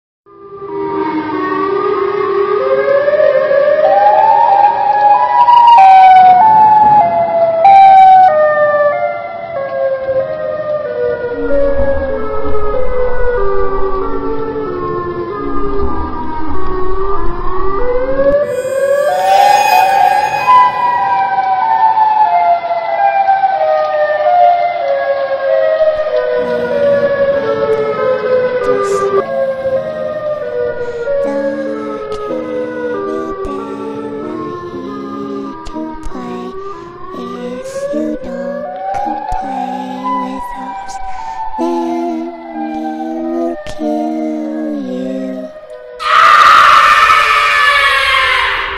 This broken siren is a thing of nightmares with a crazy twist
Chicago Has The Most Eerie Tornado Siren and zombies. It will be a fiasco.